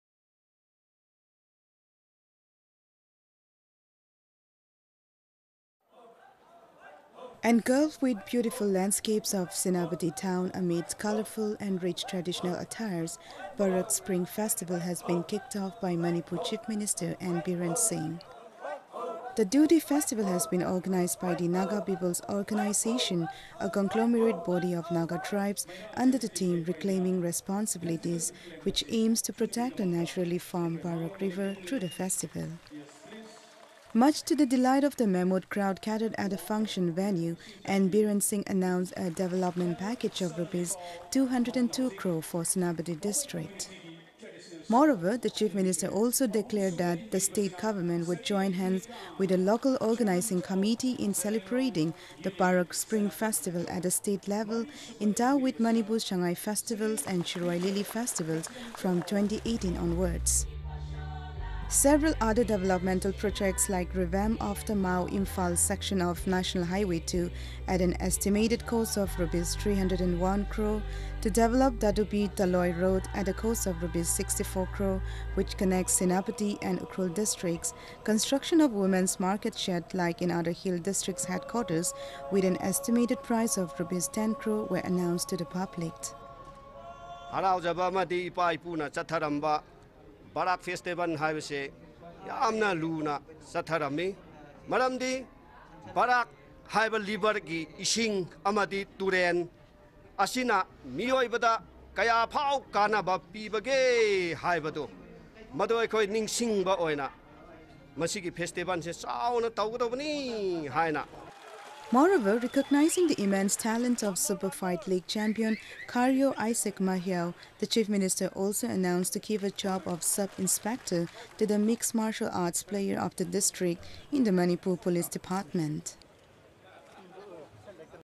Manipur CM unveils Barak Spring Festival - Manipur News
Manipur, Apr 23 (ANI): Engulfed with beautiful landscapes of Senapati town amidst colorful and rich traditional attires, Barak Spring festival has been kicked off by Manipur Chief Minister N Biren Singh. The two-day festival has been organised by the Naga People's Organisation, a conglomerate body of Naga tribes, under the theme 'Reclaiming Responsibilities' which aims to protect the naturally formed Barak River through the festival.